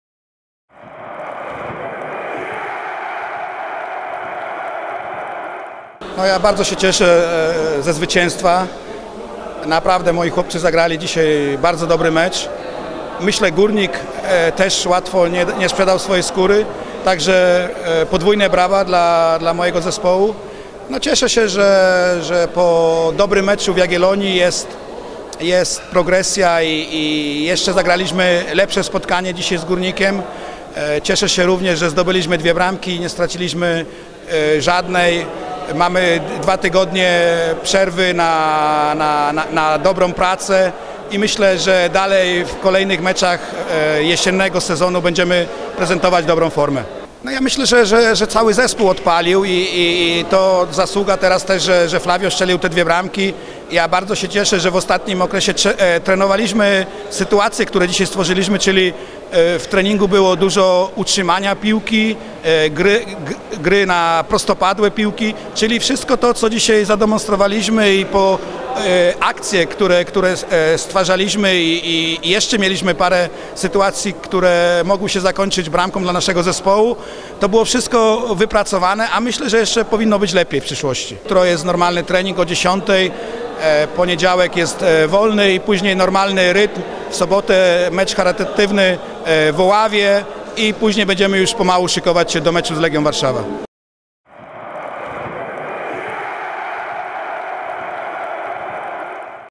Trener Pawłowski po meczu z Górnikiem Zabrze
Spotkanie dla naszej klubowej telewizji podsumował również trener Śląska Wrocław Tadeusz Pawłowski.